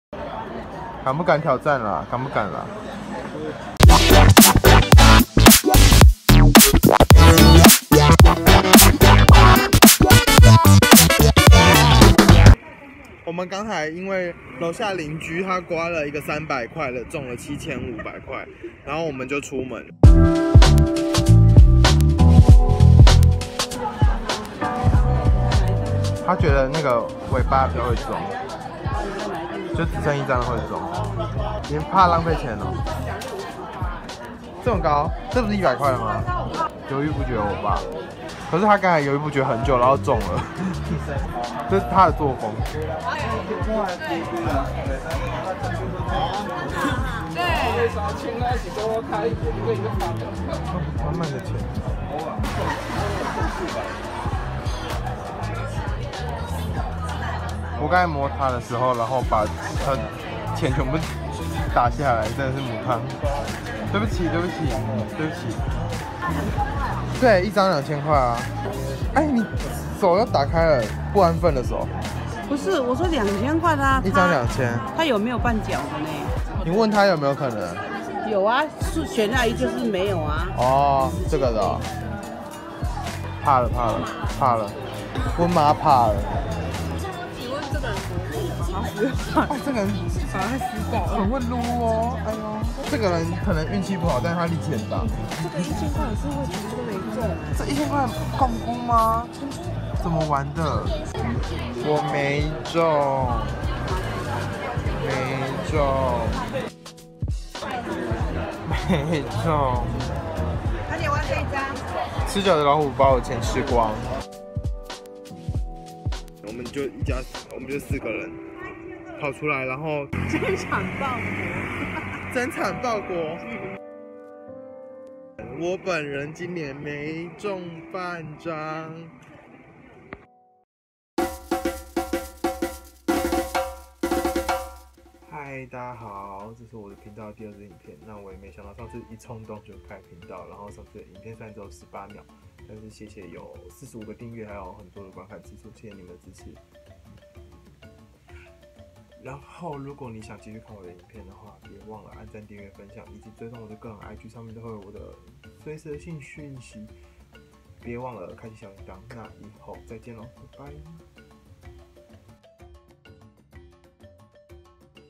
【生活#1】摸了神明刮刮樂就會中？
第一次用Premiere剪啦 好痛苦
很不會用電腦剪影片，如果大家有什麼要求或建議我都會盡量做到！